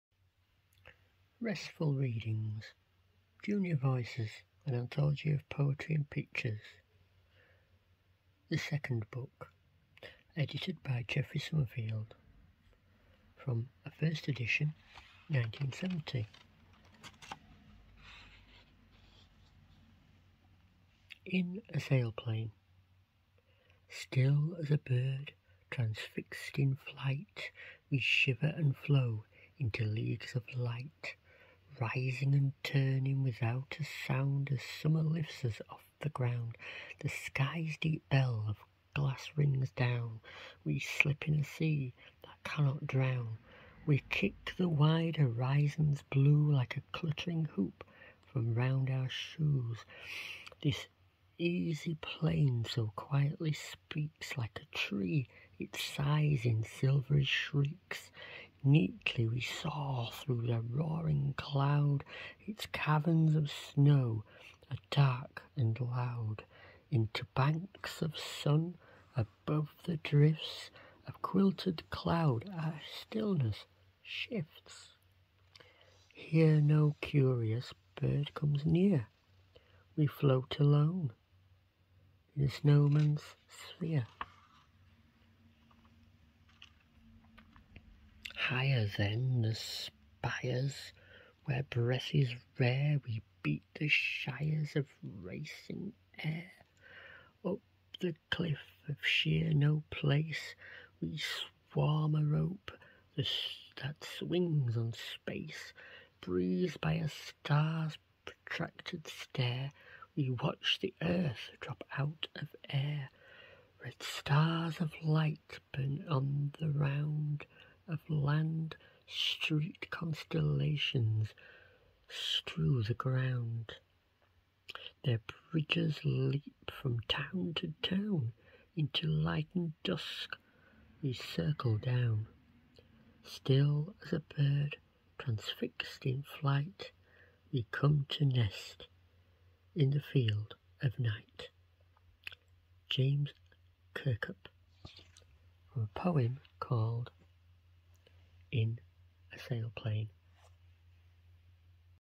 Restful readings 12 In a sailplane
Restful readings: In a sailplane.

Junior Voices, book 2,edited by Geoffrey Summerfield.